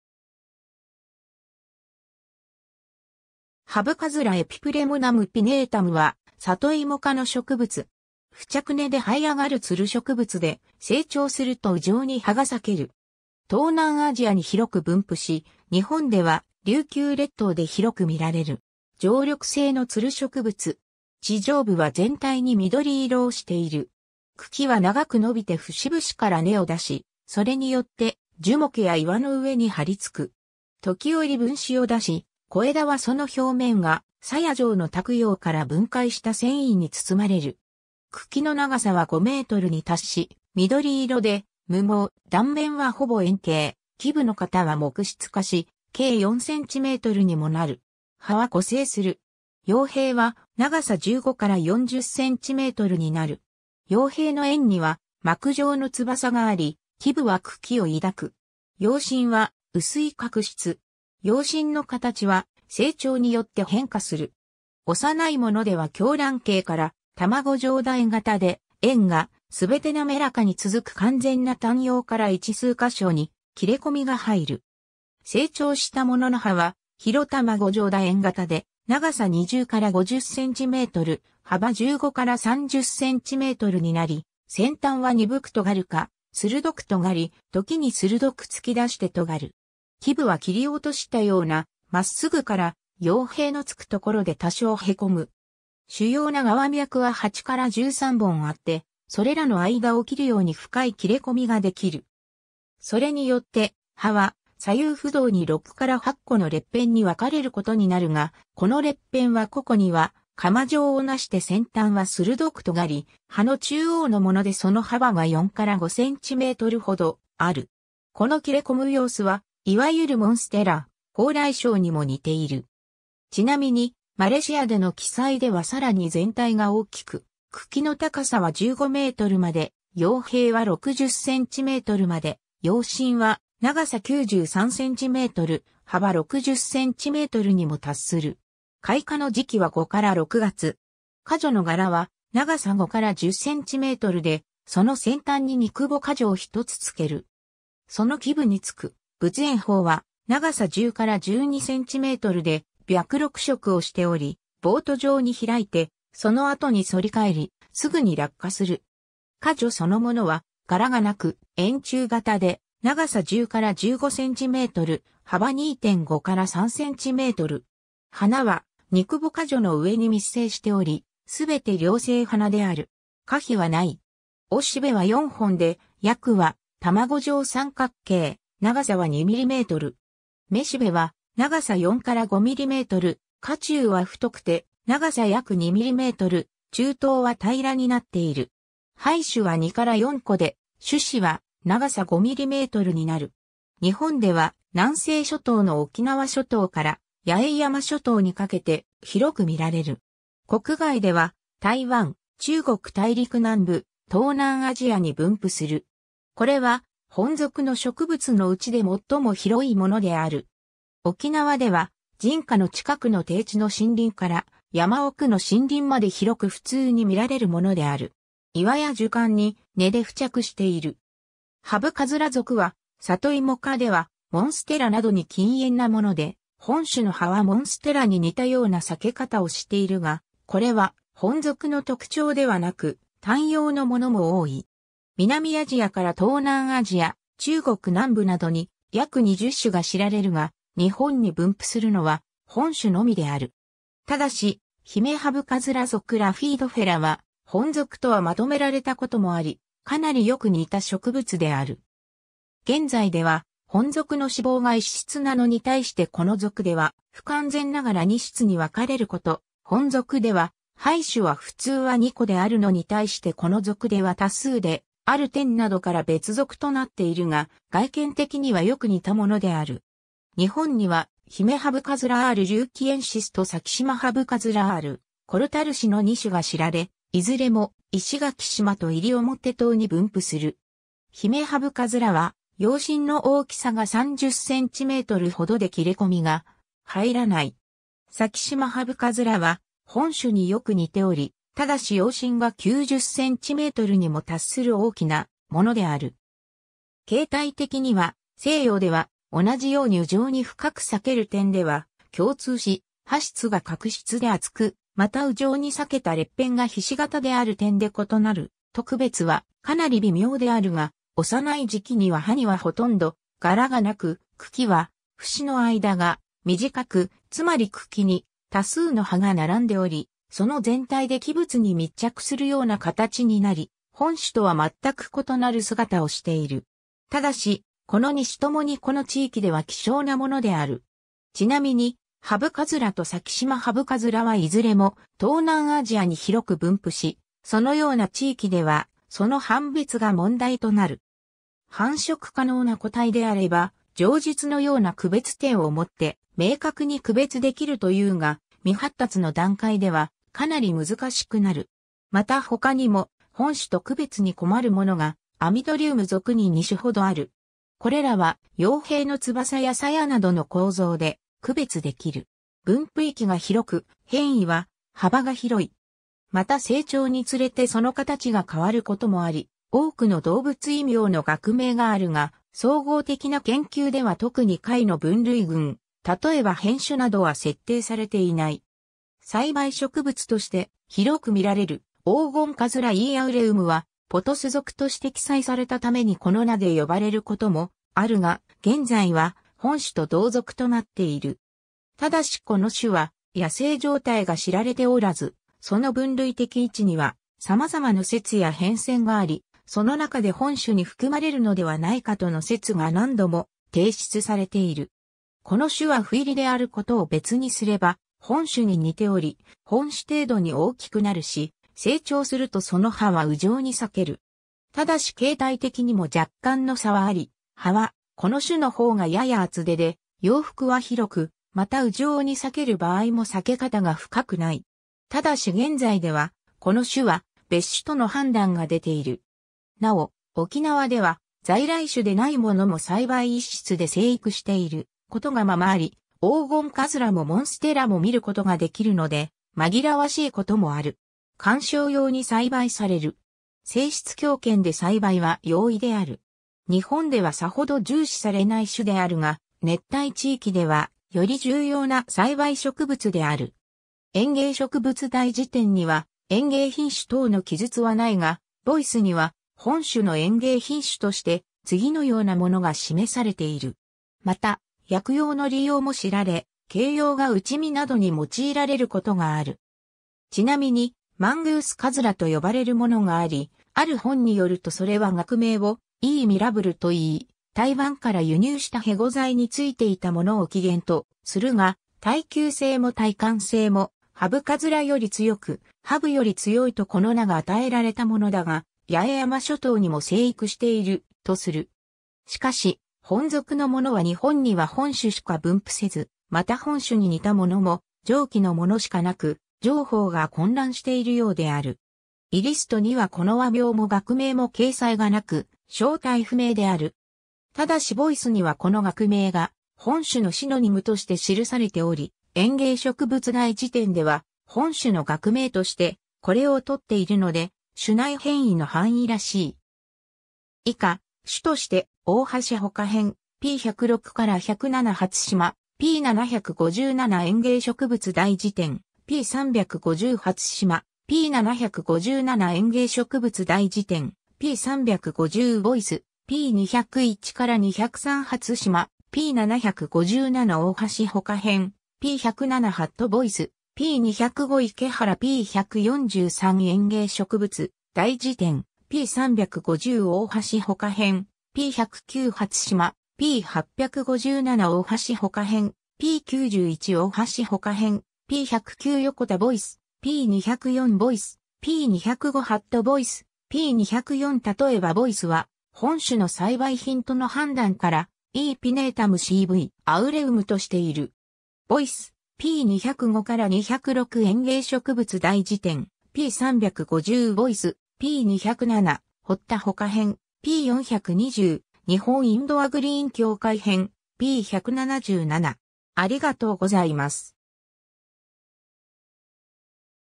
ハブカズラ
ハブカズラ Epipremnum pinnatum はサトイモ科の植物。
付着根で這い上がる蔓植物で、成長すると羽状に葉が裂ける。
東南アジアに広く分布し、日本では琉球列島で広く見られる。
 常緑性の蔓植物。
地上部は全体に緑色をしている。
茎は長く伸びて節々から根を出し、それによって樹木や岩の上に張り付く。
ときおり分枝を出し、小枝はその表面が鞘状の托葉から分解した繊維に包まれる。
茎の長さは5mに達し（10mになることもあるとも）、緑色で無毛、断面はほぼ円形、基部の方は木質化し、径4cmにもなる。
葉は互生する。
葉柄は長さ15-40cmになる。
葉柄の縁には膜状の翼があり、基部は茎を抱く。
葉身は薄い革質。
葉身の形は成長によって変化する。
幼いものでは狭卵形から卵状楕円形で、縁がすべてなめらかに続く完全な単葉から1～数カ所に切れ込みが入る。
成長したものの葉は広卵状楕円形で長さ20-50cm、幅15-30cmになり、先端は鈍く尖るか鋭く尖り、時に鋭く突き出して尖る。
基部は切り落としたような真っ直ぐから葉柄のつくところで多少凹む。
主要な側脈は8-13本あって、それらの間を切るように深い切れ込みができる。
それによって葉は左右不同に6-8個の裂片に分かれることになるが、この裂片は個々には鎌状をなして先端は鋭く尖り、葉の中央のものでその幅が4-5cmほどある。
この切れ込む様子はいわゆるモンステラ、ホウライショウにも似ている。
ちなみにマレシアでの記載ではさらに全体が大きく、茎の高さは15mまで、葉柄は60cmまで、葉身は長さ93cm、幅60cmにも達する。
 開花の時期は5-6月。
花序の柄は長さ5-10cmで、その先端に肉穂花序を1つつける。
その基部に着く仏炎苞は長さ10-12cmで白緑色をしており、ボート状に開いて、その後に反り返り、すぐに落下する。
花序そのものは柄がなく、円柱形で長さ10-15cm、幅2.5-3cm。
花は肉穂花序の上に密生しており、すべて両性花である。
花被はない。
雄しべは4本で葯は卵状三角形、長さは2mm。
雌しべは長さ4-5mm、花柱は太くて長さ約2mm、柱頭は平らになっている。
胚珠は2-4個で、種子は長さ5mmになる。
 日本では南西諸島の沖縄諸島から八重山諸島にかけて広く見られる。
国外では台湾、中国大陸南部、東南アジアに分布する。
これは本属の植物のうちで最も広いものである。
 沖縄では人家の近くの低地の森林から山奥の森林まで広く普通に見られるものである。
岩や樹幹に根で付着している。
 ハブカズラ属はサトイモ科ではモンステラなどに近縁なもので、本種の葉はモンステラに似たような裂け方をしているが、これは本属の特徴ではなく、単葉のものも多い。
南アジアから東南アジア、中国南部などに約20種が知られるが、日本に分布するのは本種のみである。
ただし、ヒメハブカズラ属 Rhaphidophora は本属とはまとめられたこともあり、かなりよく似た植物である。
現在では本属の子房が1室なのに対してこの属では不完全ながら2室に分かれること、本属では胚珠は普通は2個(時に8個まで)であるのに対してこの属では多数である点などから別属となっているが、外見的にはよく似たものである。
日本にはヒメハブカズラ R. liukiuensis とサキシマハブカズラ R. korthalsii の2種が知られ、いずれも石垣島と西表島に分布する。
ヒメハブカズラは葉身の大きさが30cmほどで切れ込みが入らない。
サキシマハブカズラは本種によく似ており、ただし葉身が90cmにも達する大きなものである。
形態的には成葉では同じように羽状に深く裂ける点では共通し、葉質が革質で厚く、また羽状に裂けた裂片が菱形である点で異なる、と区別はかなり微妙であるが、幼い時期には葉にはほとんど柄がなく、茎は節の間が短く、つまり茎に多数の葉が並んでおり、その全体で基物に密着するような形になり、本種とは全く異なる姿をしている。
ただし、この2種ともにこの地域では稀少なものである。
 ちなみにハブカズラとサキシマハブカズラはいずれも東南アジアに広く分布し、そのような地域ではその判別が問題となる。
繁殖可能な個体であれば上述のような区別点をもって明確に区別できるというが、未発達の段階ではかなり難しくなる。
またほかにも本種と区別に困るものが Amydrium 属に2種ほどある。
これらは葉柄の翼や鞘などの構造で区別できる。
 分布域が広く、変異は幅が広い。
また成長につ...